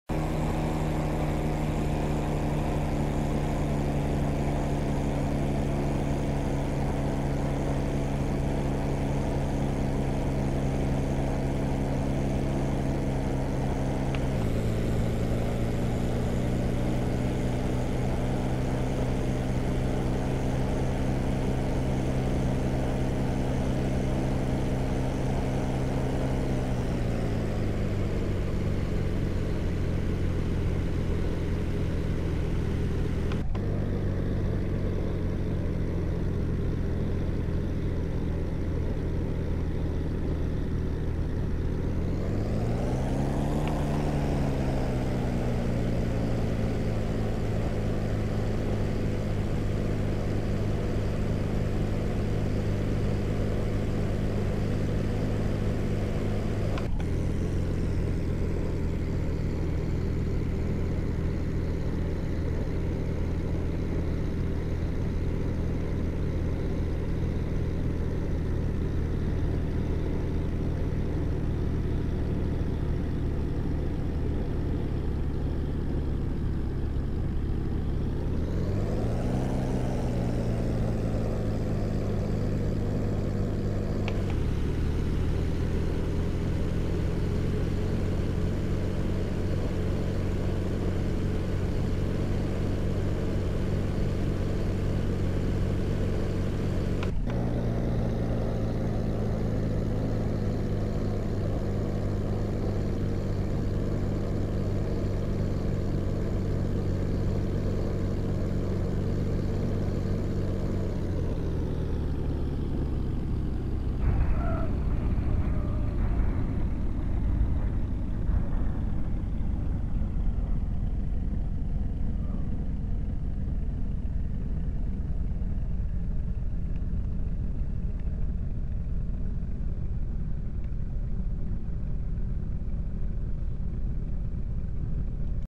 P92 atterragio a Castelfranco
Atterraggio al campo volo Rotocenter a Castelfranco di sotto (Pisa) .
Simula fedelmente i reali atterraggi che ho fatto per tanti anni quando dalle marche andavo a trovare i miei figli che stanno li vicino, quindigarantisco personalmente il realismo. se volete provare ance voi questo simulatore dal VFR estremo prenotate un appuntamento al cell.3392006705